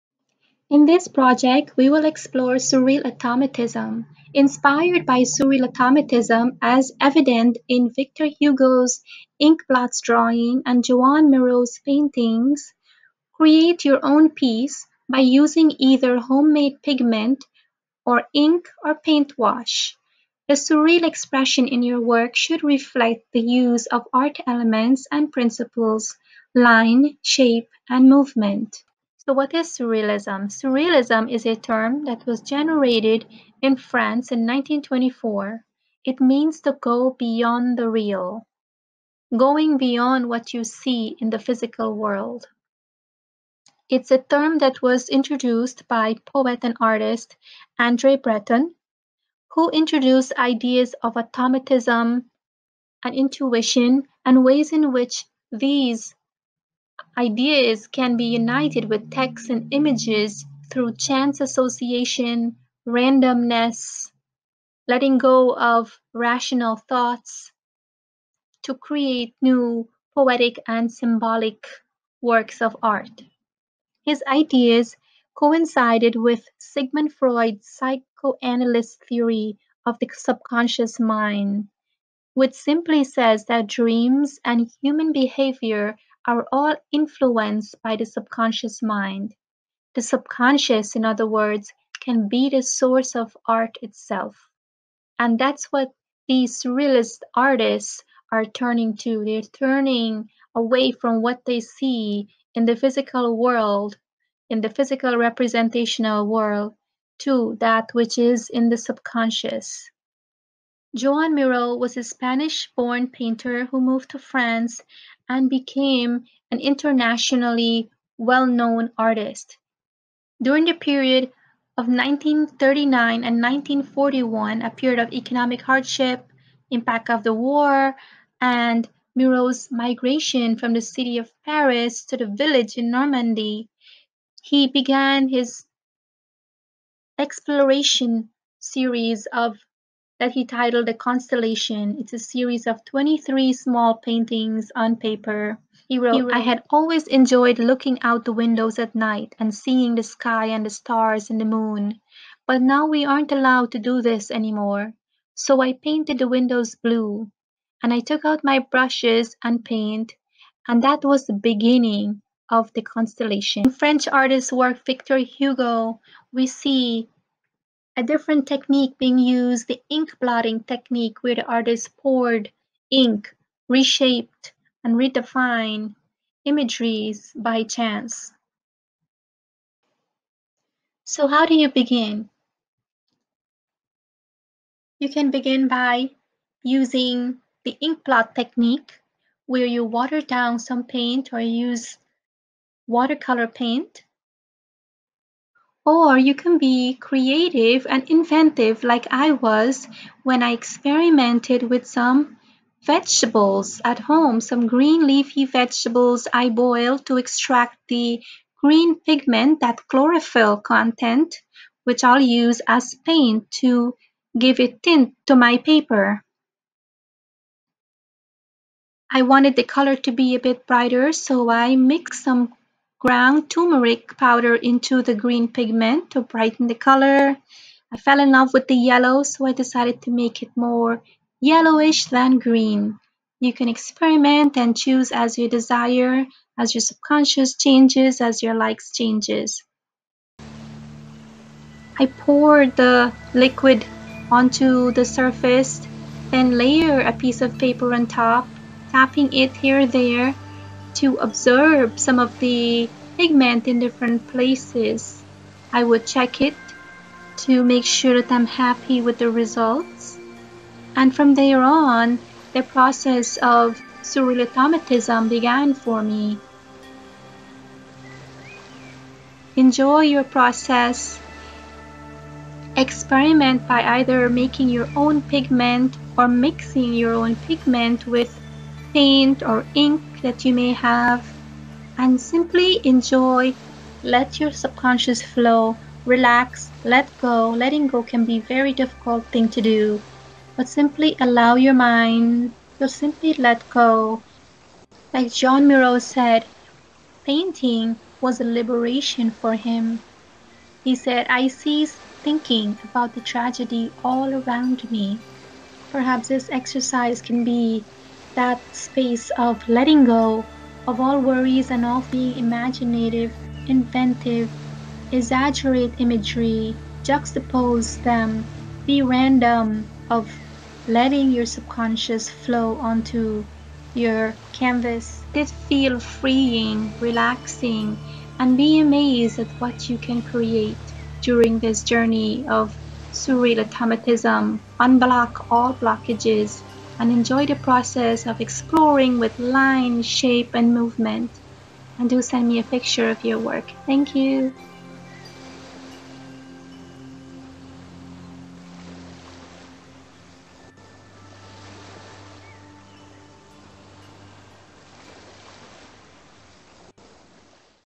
Surreal Automatism_Joan Miro_Distance Learning_Ms Pertab
Inspired by Surreal Automatism, students will create a drawing or painting that expresses their subconscious by applying line, shape and movement. In this example, I shared some inspiration to unblock the rational mind and ways common pigment from nature can be used as ink to add color to the canvas or paper surface. Enjoy  the process...and become inspired to create your own surreal painting.